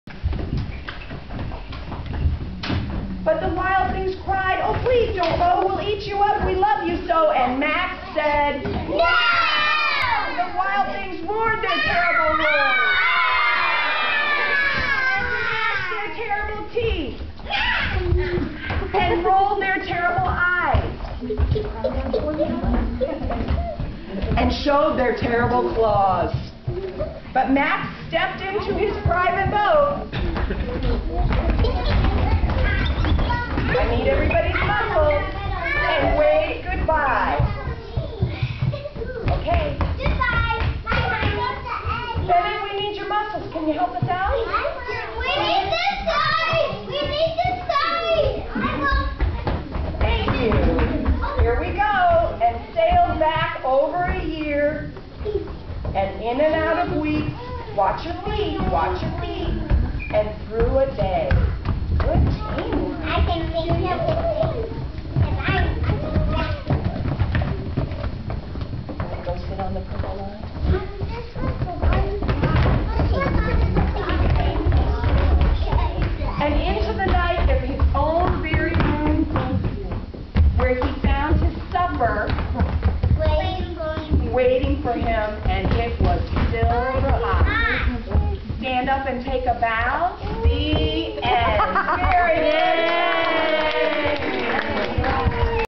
COCA performance Part 3
COCA spring break camp. "Where the Wild Things Are" performance.  PART 3